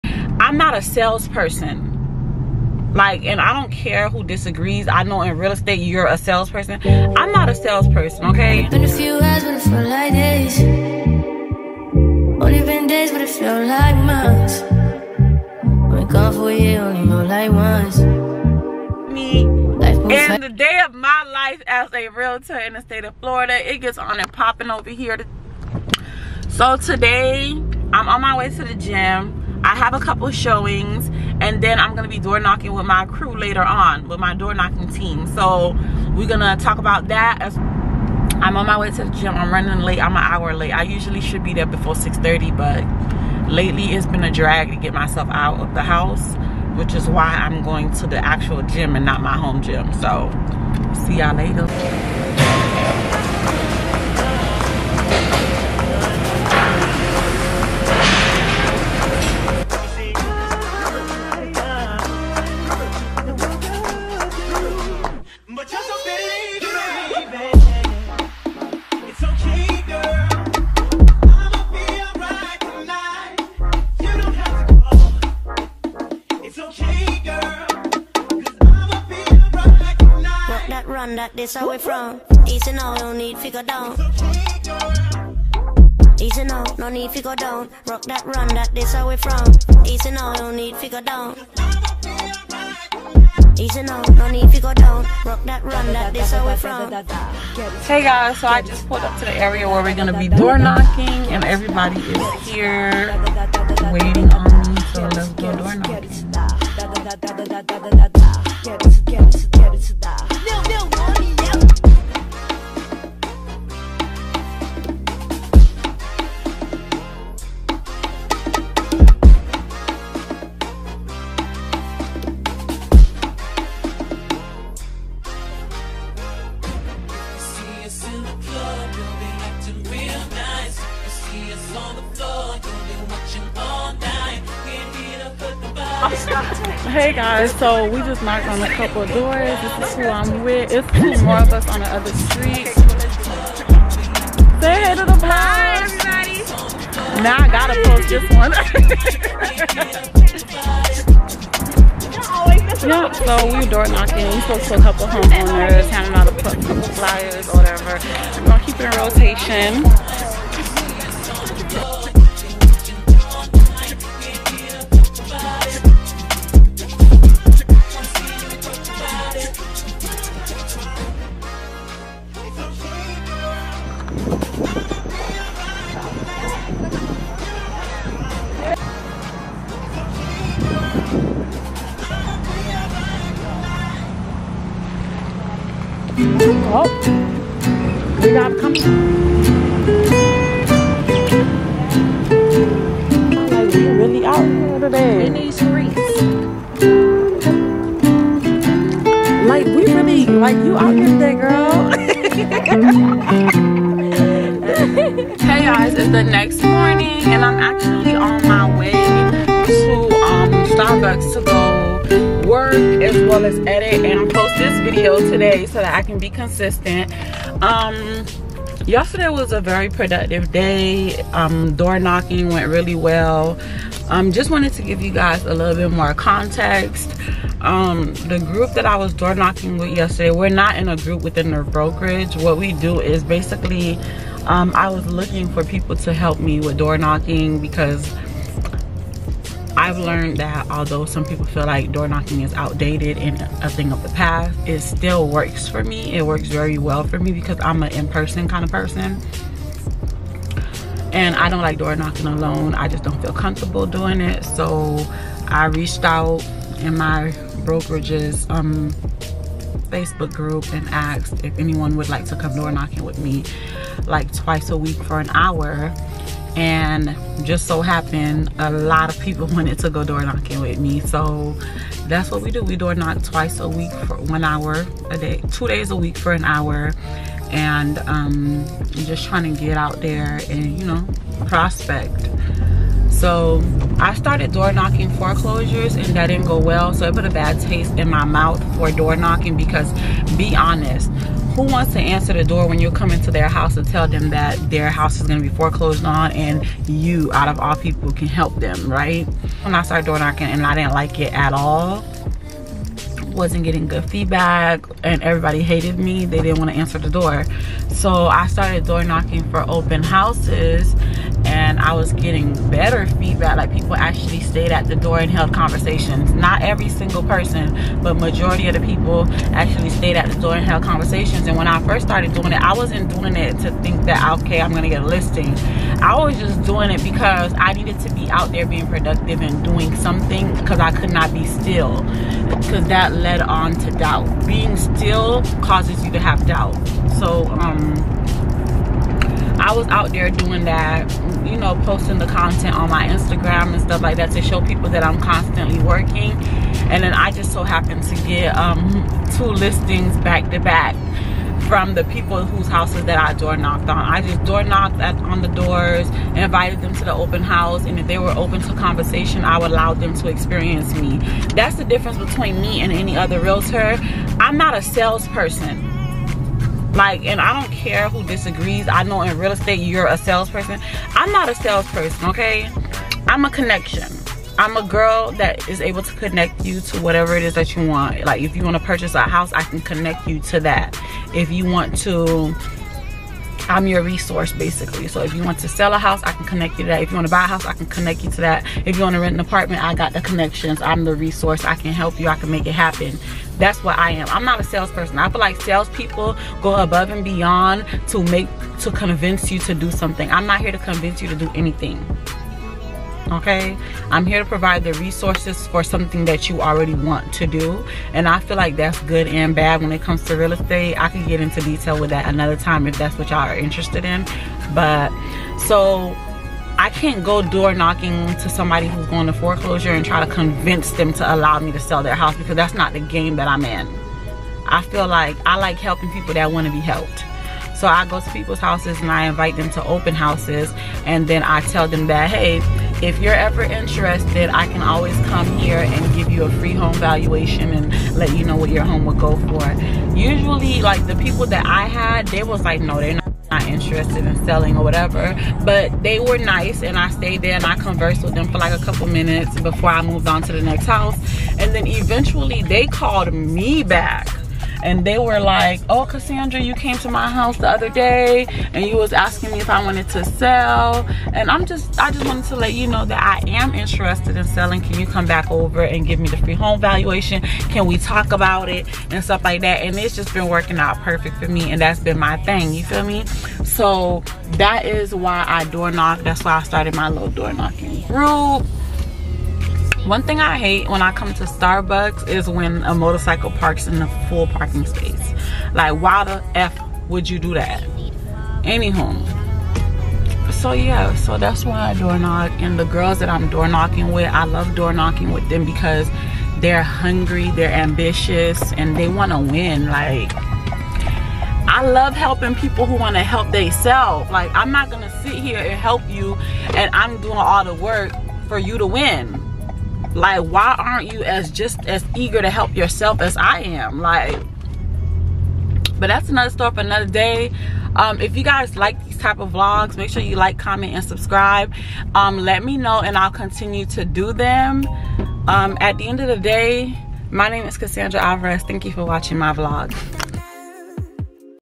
Day in the Life of a Realtor: Going Door knocking and How I get Listings
In this video, we'll take a look at how I go about door knocking and how I get listings. By taking this approach, we can get more listings for our clients and reduce the time it takes to sell a property.

If you're looking to get more listings for your real estate business, then check out this day in the life of a Realtor! In this video, we'll go over the different steps we take to get listings and how we can use door knocking to get the best results for our clients.
Hey guys in this video I show you a day in the life of a realtor. Gym Vlog and going door knocking I also tell you how I get my listings .